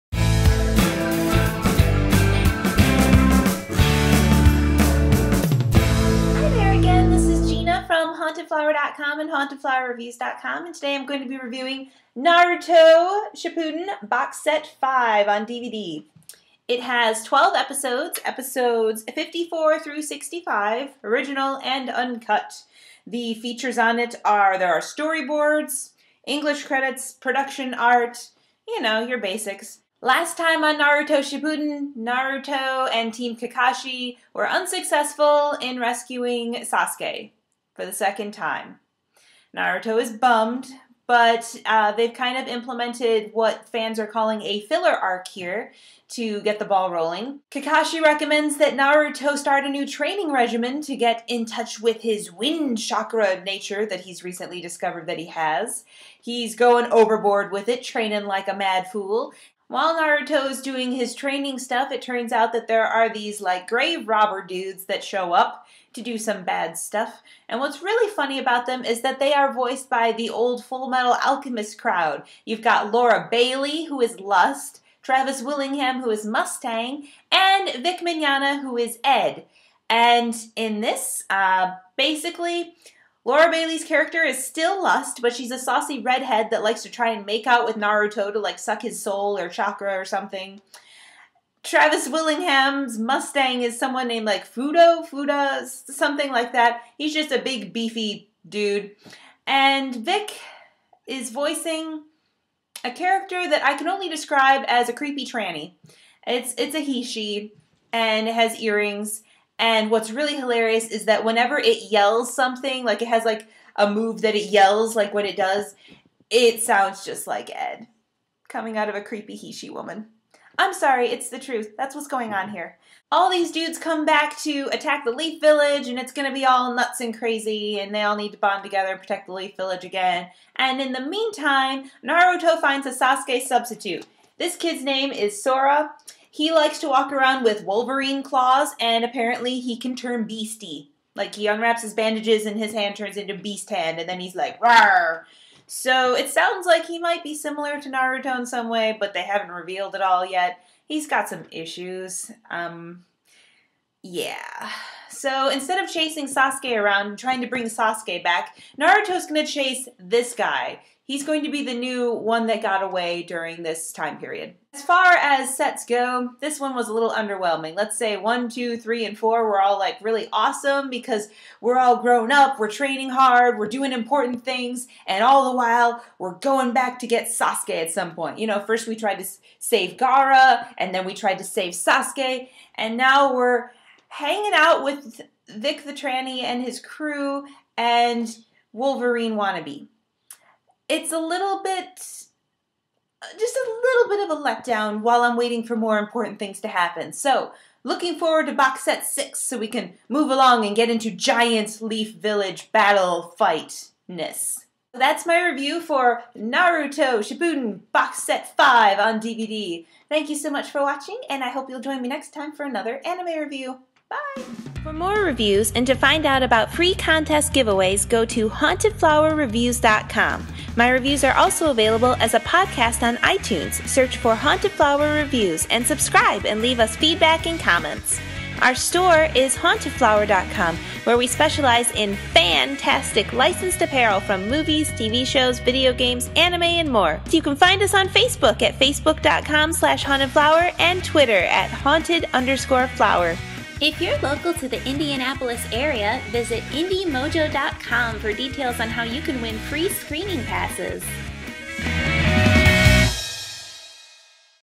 Naruto Shippuden Box Set 5 Review from Haunted Flower
Gina from HauntedFlower.com and HauntedFlowerreviews.com reviews the next installment in the anime, Naruto Shippuden - Box Set 5 on DVD!